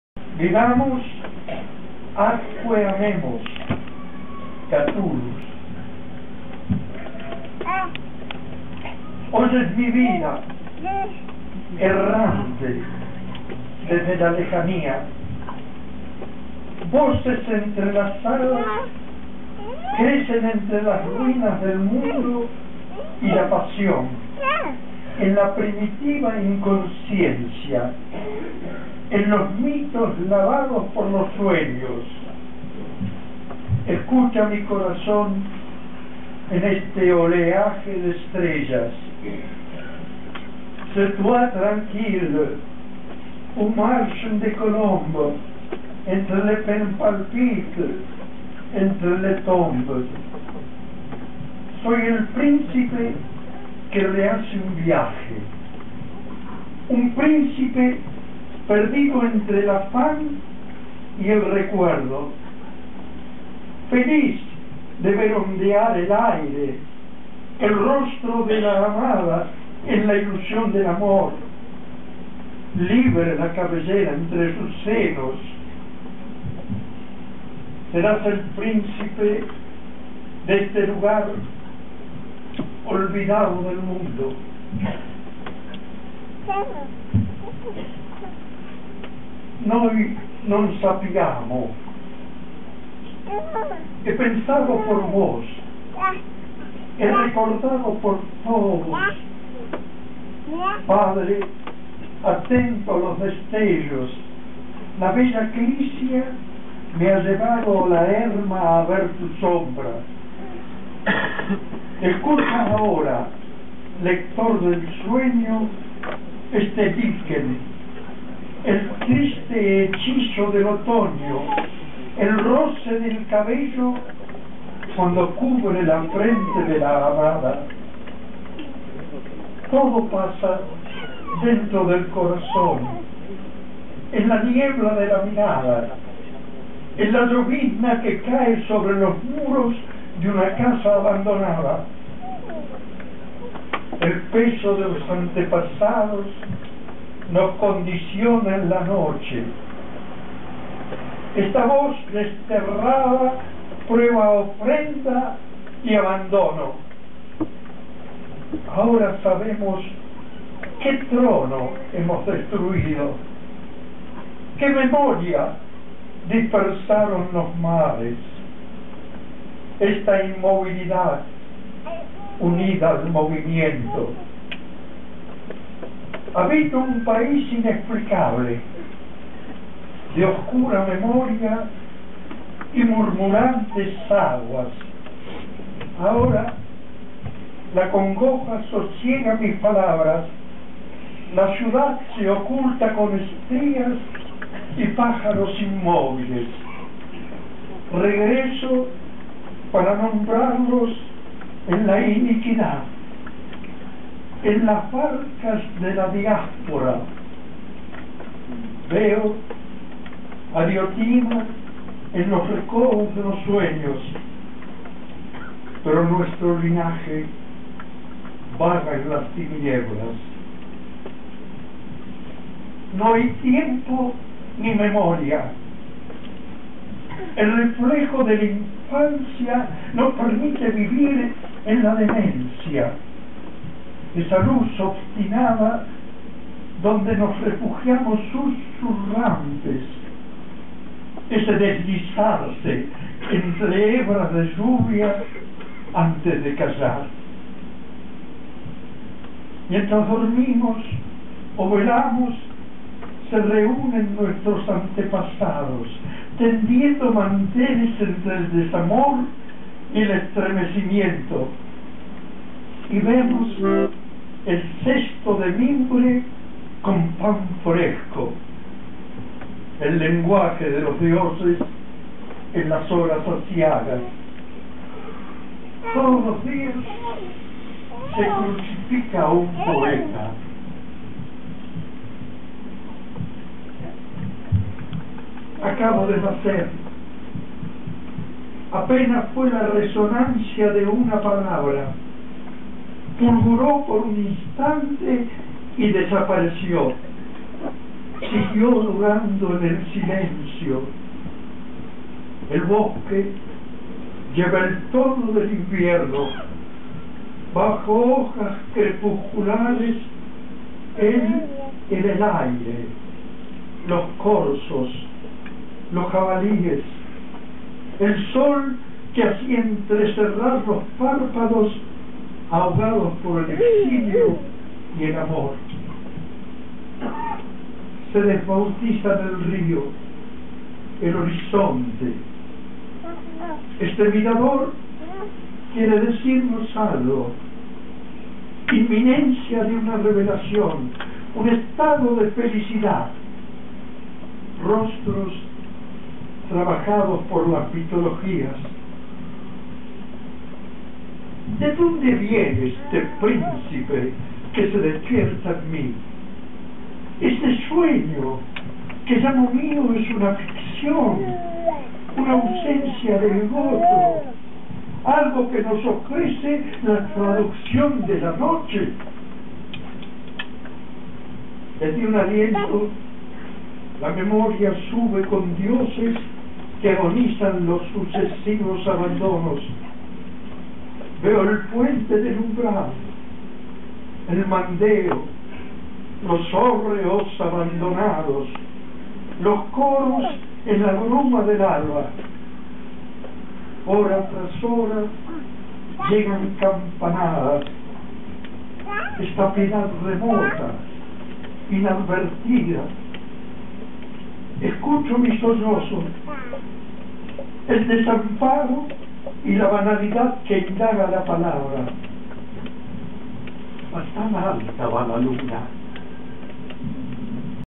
Romancero de la melancolía, nuevo libro de Carlos Penelas
Onofre Lovero lee partes del nuevo libro de Carlos Penelas, "Romancero de la melancolía" (2007, Centro Betanzos Ediciones), en la presentación que se hizo en el Centro Betanzos de Buenos Aires, junto a Xeito Novo, el 23 de junio de 2007. Parte 3 de 7.